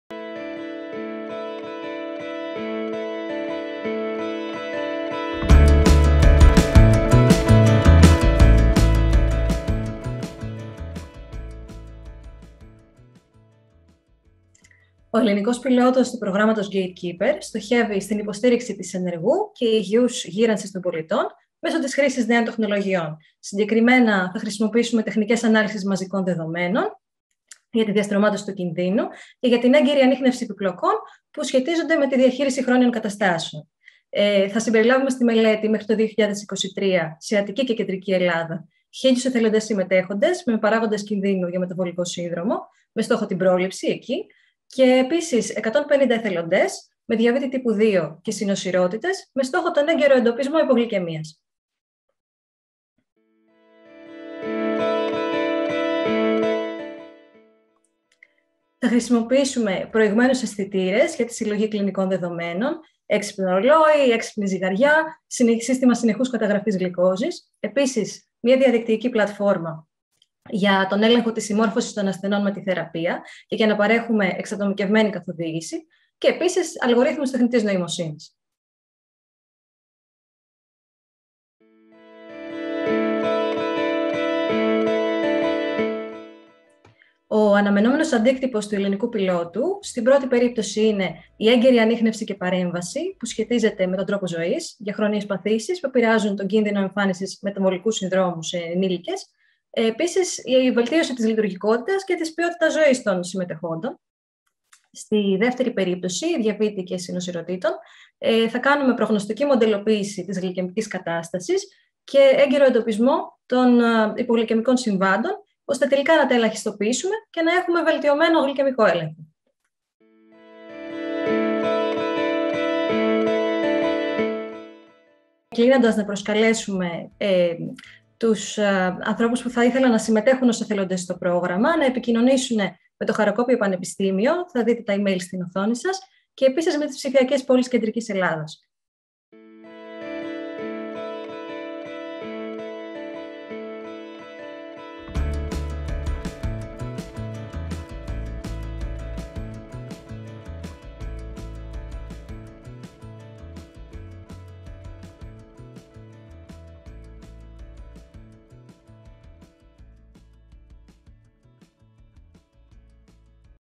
Attica and Central Greece Pilot Overview
A short video providing an overview of the Attica and Central Greece pilot covering:
 - The main objectives
 - The technologies in use
 - The expected impact